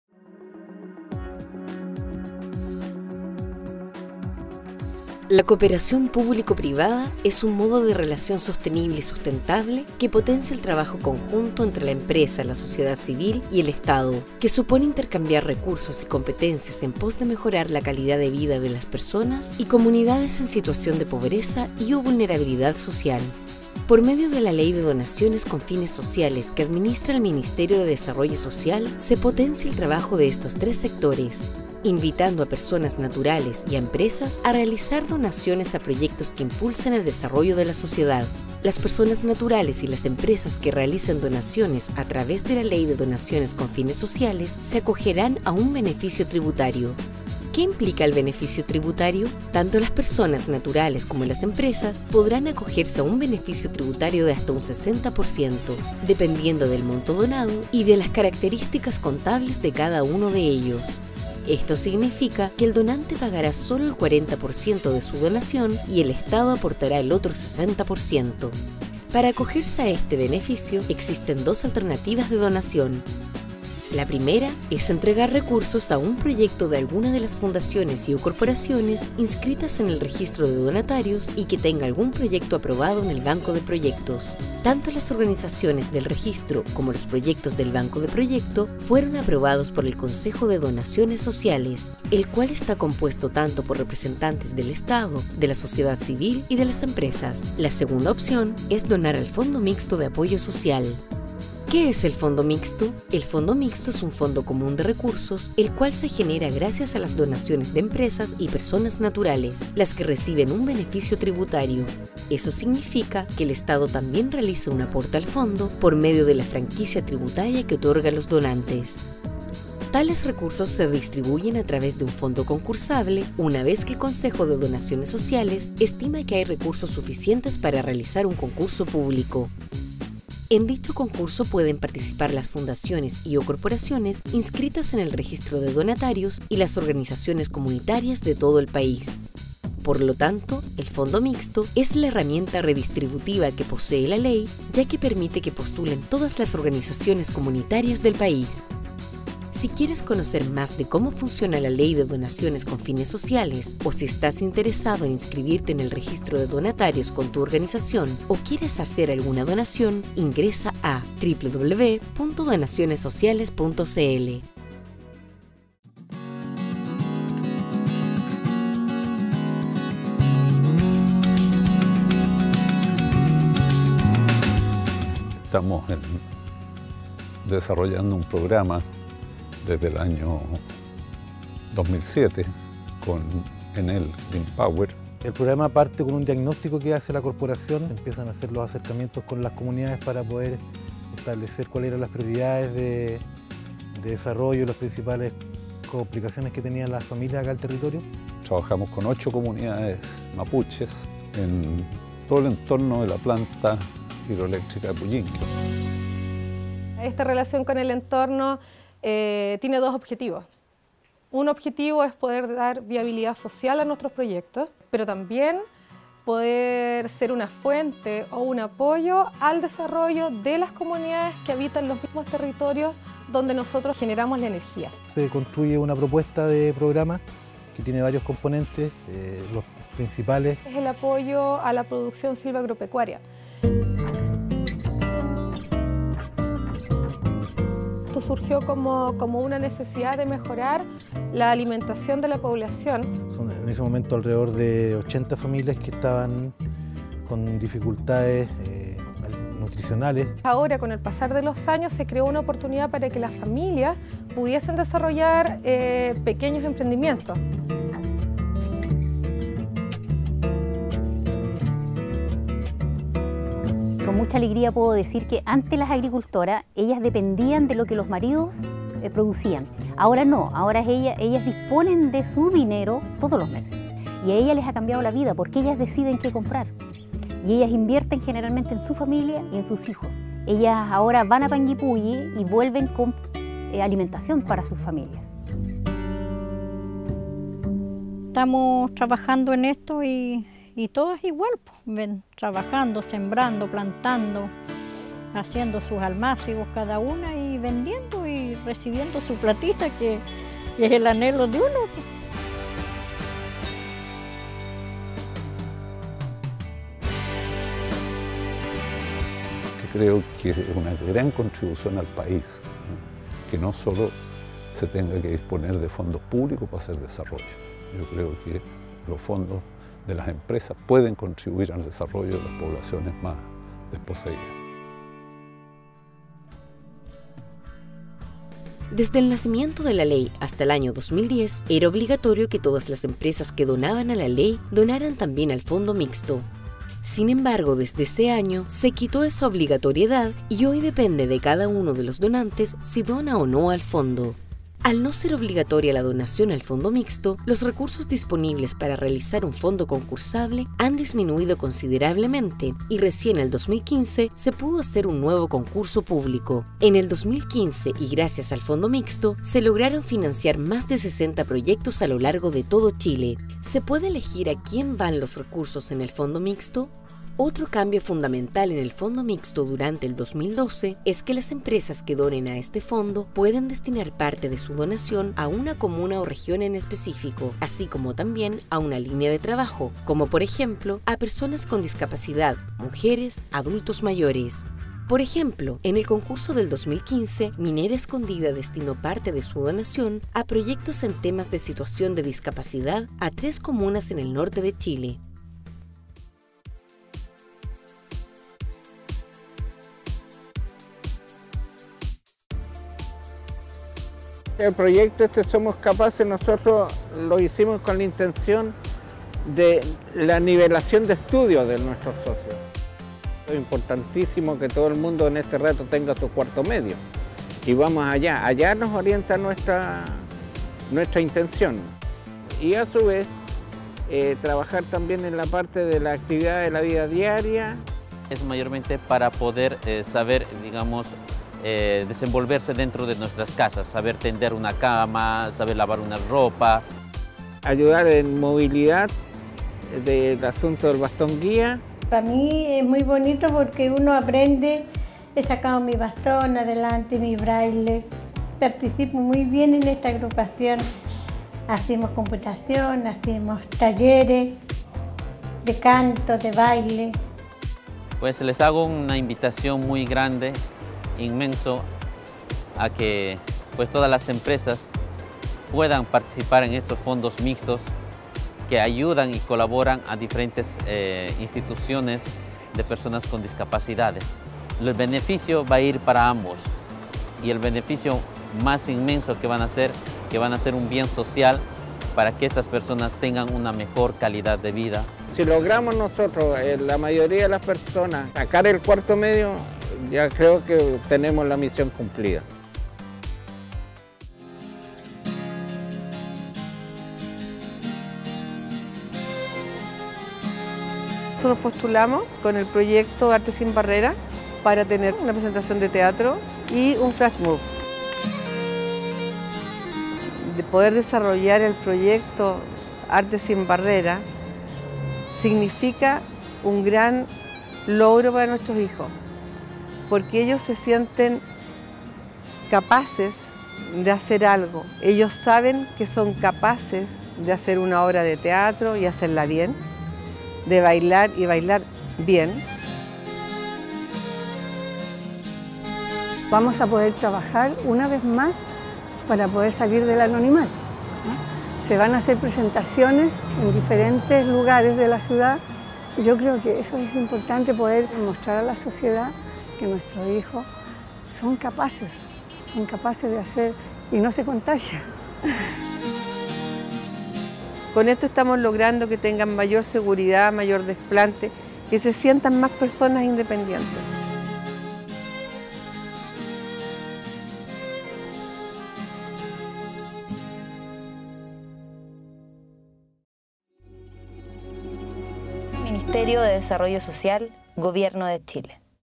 Ley de Donaciones con Fines Sociales con casos de proyectos financiados
La Ley N° 19.885, más conocida como Ley de Donaciones con Fines Sociales, es una herramienta de Cooperación Público Privada que incentiva las donaciones de empresas y personas naturales a organizaciones sociales sin fines de lucro que tengan como objetivo contribuir a mejorar la calidad de vida de las personas y/o comunidad en situación de pobreza y/o de discapacidad, así como de prevención y rehabilitación de alcohol y/o drogas.
Conoce los detalles de la Ley, sus principales características y casos de proyectos que han financiado gracias a ésta.